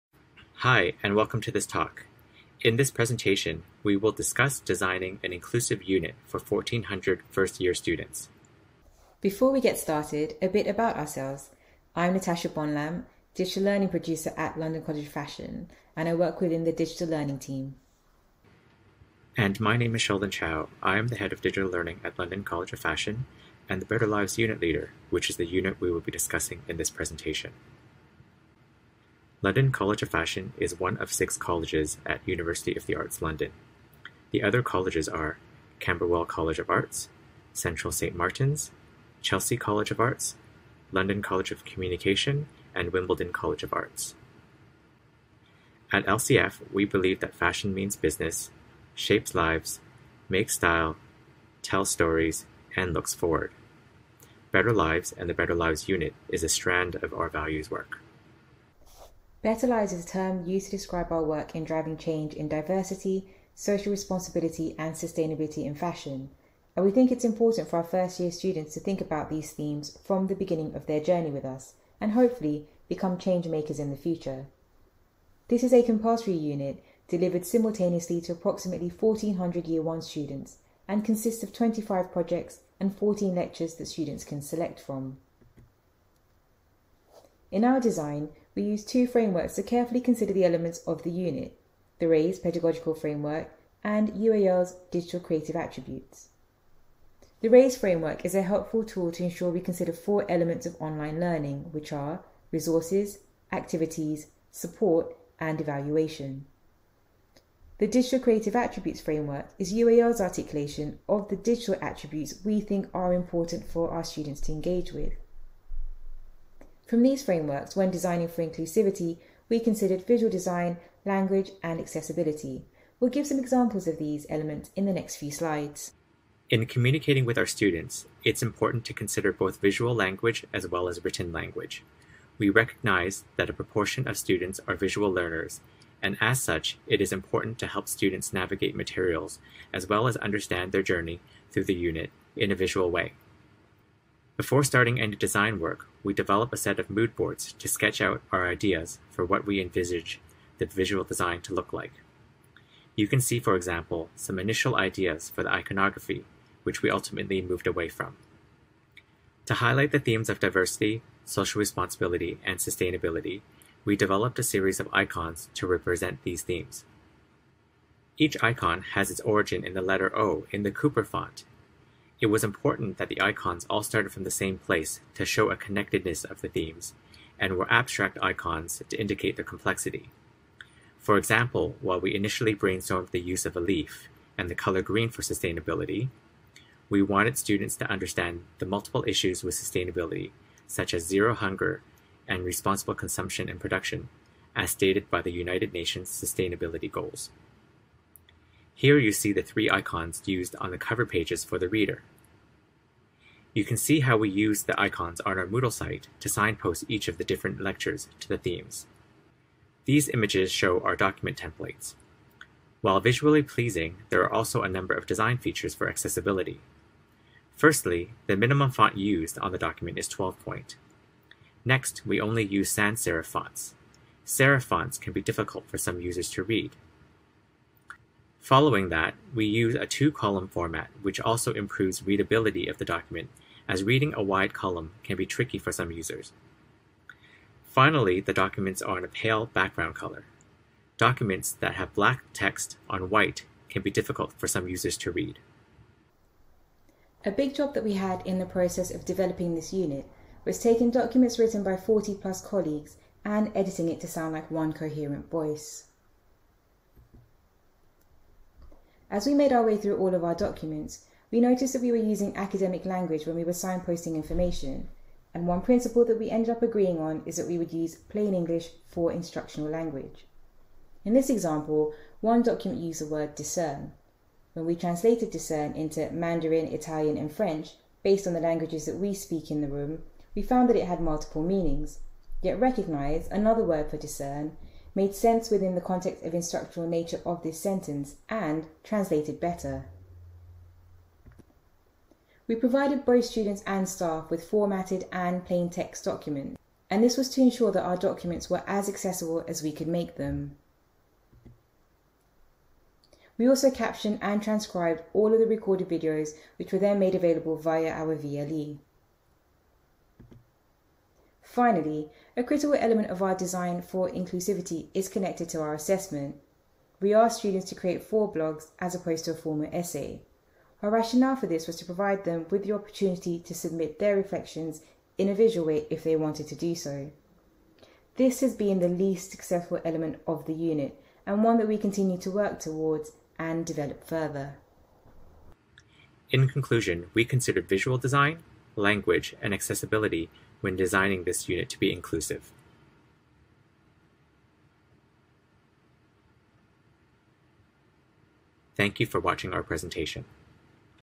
Designing an inclusive unit for 1400 first year students [A-059]
Video CC-BY: Sheldon Chow and Natasha Bonnelame

In 2019, London College of Fashion launched a new first year unit called Better Lives that introduces students to themes of diversity, social responsibility and sustainability in fashion. The unit is a compulsory component of student’s first year and is delivered to the entire cohort which is approximately 1400 students.

As part of this development and considering the large number and diverse nature of our student body, inclusive design was an important aspect to ensure students could access, navigate and comprehend the information and activities being presented to them.

Starting with the RASE pedagogical framework we mapped all the components of our online resources/activities and considered how we needed to adapt the visual nature, language and medium. In this talk we will give a quick overview of some of these considerations and how we integrated students into the design process.